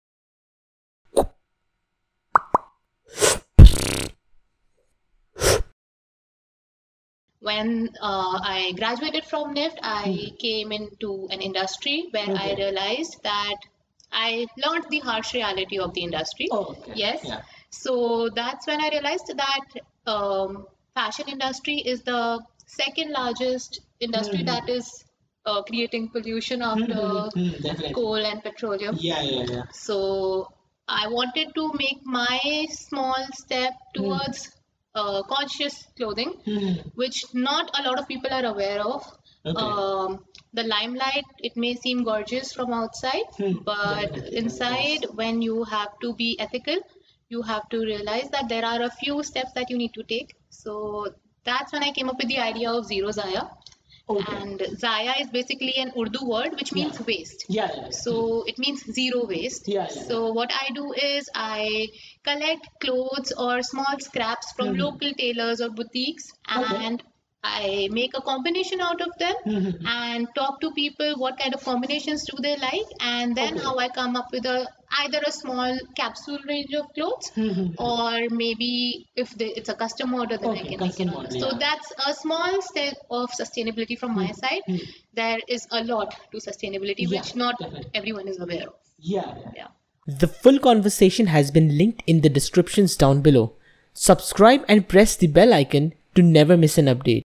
Building a Sustainable Fashion Brand ( Zero Zaya ) | The Design Baithak | Ft. Irum Khan
Irum Khan is an alumni from NIFT Delhi who has graduated in the field of fashion design. Her interest in Design started from her habits of the back page scribble and her journey continues to becoming the founder of a sustainable fashion brand. In this conversation, she talks about her experiences in NIFT as a student and how every assignment contributed towards helping her build an identity. She also talks about how parents play an important role in everyone's life. Moreover she talks about her experience in an Export house where she discovered her interests in sustainability and how that word is exploited by brands to market the products. She includes 'Zero Zaya' and her ideologies in the conversation where the importance of upcycling is talked about.